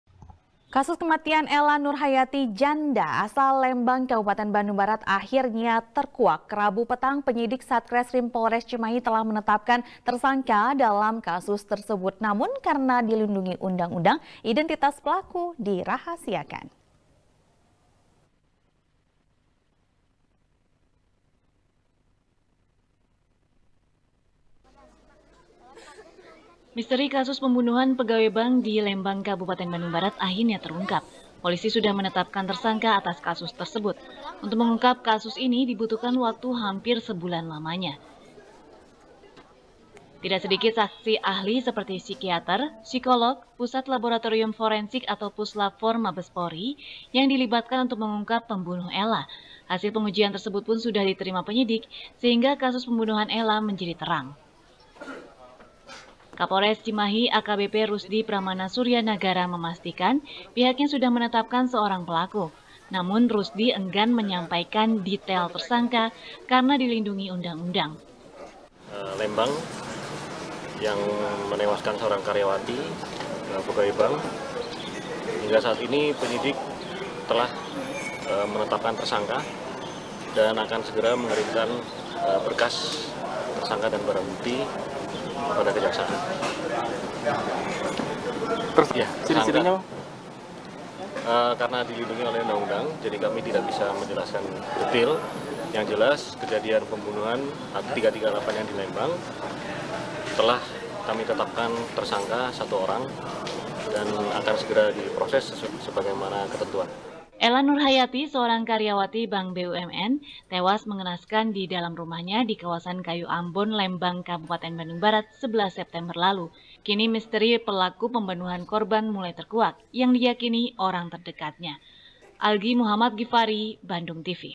Polisi Tetapkan Tersangka Pembunuh Karyawati Bank BUMN | SBR | BANDUNGTV
SBR | BANDUNGTV | KRISANTI | Kasus kematian ella nurhayati, janda asal lembang kabupaten bandung barat, akhirnya terkuak. rabu petang, penyidik satreskrim polres cimahi telah menetapkan tersangka dalam kasus tersebut. namun, karena dilindungi undang-undang, indentitas pelaku dirahasiakan.